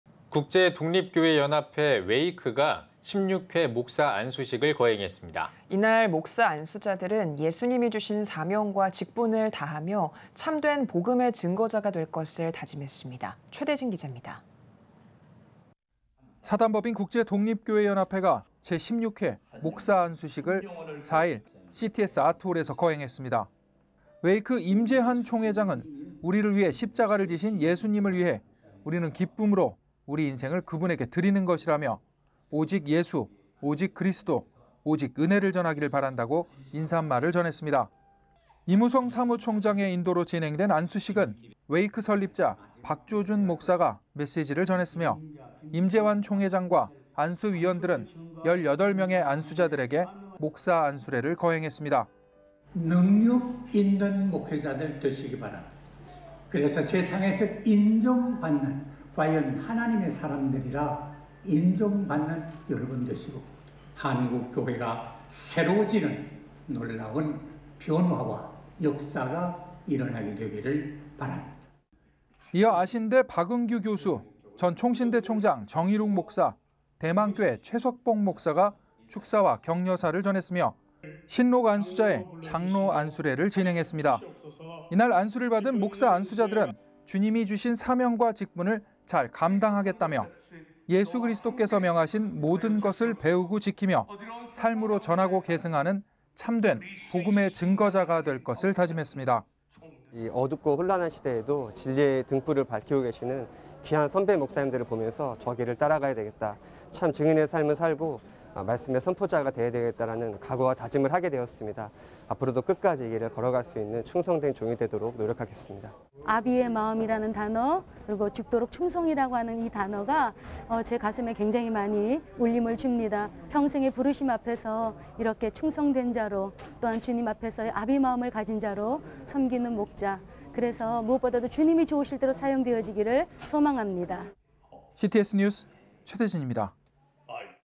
웨이크 제16회 목사안수식, “참된 복음 증거자 되자”(최대진) l CTS뉴스
앵커 : 사단법인 국제독립교회연합회인 웨이크가 제16회 목사안수식을 거행했습니다.

앵커 : 이날 목사안수자들은 주님이 주신 사명과 직분을 잘 감당해 참된 복음의 증거자가 될 것을 다짐했는데요. 최대진 기잡니다.

사단법인 국제독립교회연합회가 제16회 목사안수식을 4일 CTS아트홀에서 거행했습니다.
웨이크 임재환 총회장은 “우리를 위해 십자가를 지신 예수님을 위해 우리는 기쁨으로 우리 인생을 그분에게 드리는 것”이라며, “오직 예수, 오직 그리스도, 오직 은혜를 전하길 바란다”고 인사말을 전했습니다.

임우성 사무총장의 인도로 진행된 안수식은 웨이크 설립자 박조준 목사가 메시지를 전했으며, 임재환 총회장과 안수위원들은 18명의 안수자들에게 목사안수례를 거행했습니다.

SOT 박조준 목사 / 사)국제독립교회연합회 설립자
능력있는 목회자들 되시기 바랍니다 그래서 세상에서 인정받는 하나님의 사람들로 인정받는 여러분 되시고 한국교회가 새로워지는 놀라운 변화와 역사가 일어나게 되길 바랍니다

이어 아신대 박응규 교수, 전 총신대 총장 정일웅 목사, 대망교회 최석봉 목사가 축사와 격려사를 전했으며, 신 록 안수자의 장로안수례를 진행했습니다.

이날 안수를 받은 목사안수자들은 “주님이 주신 사명과 직분을 잘 감당하겠다”며, 예수 그리스도께서 명하신 모든 것을 배우고 지키며, 삶으로 전하고 계승하는 참된 복음의 증거자가 될 것을 다짐했습니다.

INT 홍성준 목사안수자 / 사)국제독립교회연합회, 길위에소리교회
어둡고 혼란한 시대에도 진리의 등불을 밝히고 계시는 귀한 선배 목사님들을 보면서 저 길을 따라가야겠다 참 증인의 삶을 살고 말씀의 선포자가 되어야겠다는 각오와 다짐을 했습니다 앞으로 끝까지 이 길은 걸어가는 충성된 종이 되도록 노력하겠습니다

INT 정유경 목사안수자 / 사)국제독립교회연합회, 모현서부영락교회
아비의 마음이란 단어와 죽도록 충성이라는 단어가 제 가슴에 큰 울림을 줍니다 평생의 부르심 앞에서 충성된 자로 하나님 앞에서 아비의 마음을 가진 자로 섬기신 목자 무엇보다 주님이 좋으시게 사용되어지길 소망합니다

CTS뉴스 최대진입니다.